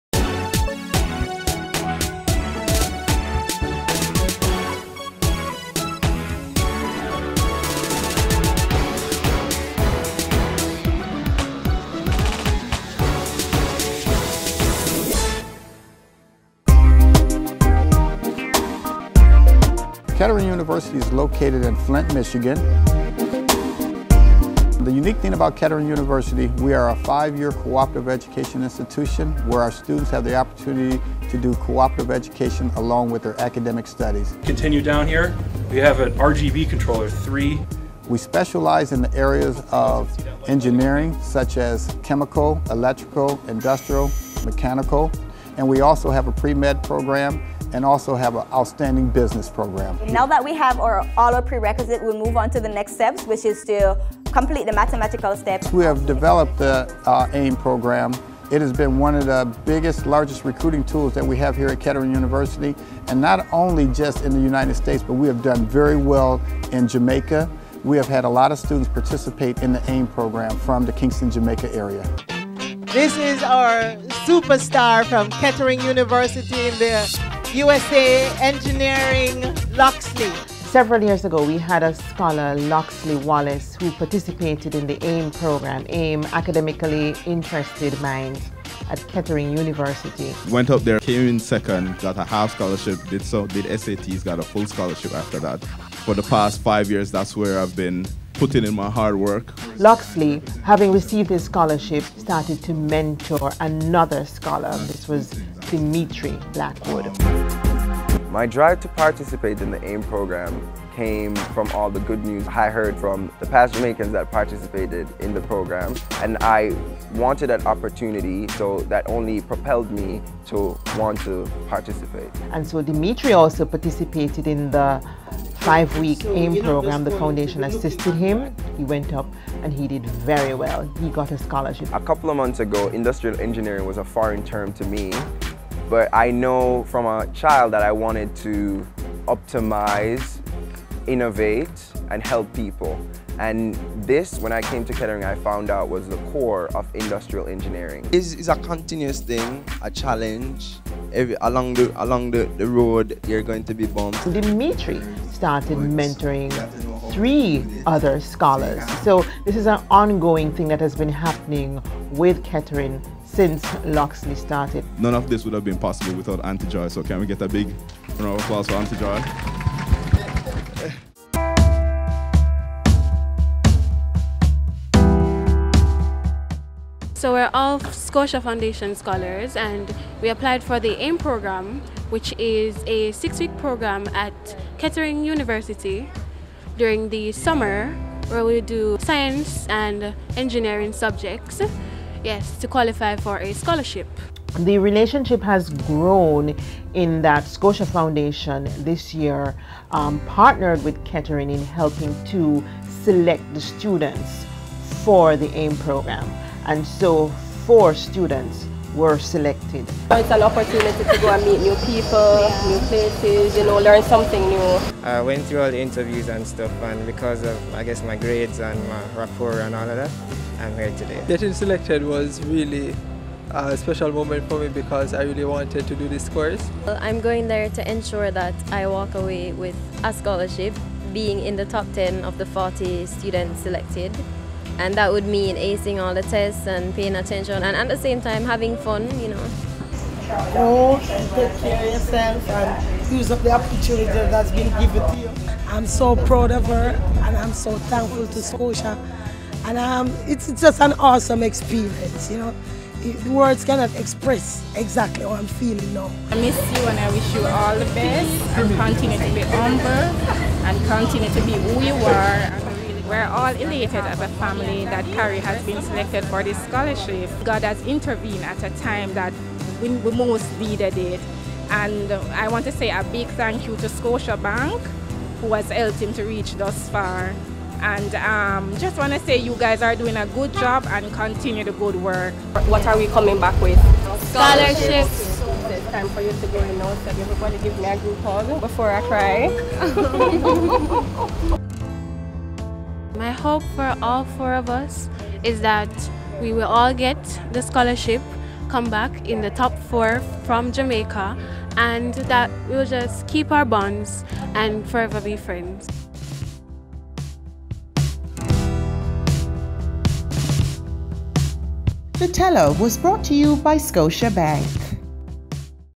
Scotiabank, The Teller Series, 2015, Episode 3 - Kettering University pt 1
See Part 1 of a 2 part feature on four ScotiaScholars who participated in the AIM Program at Kettering University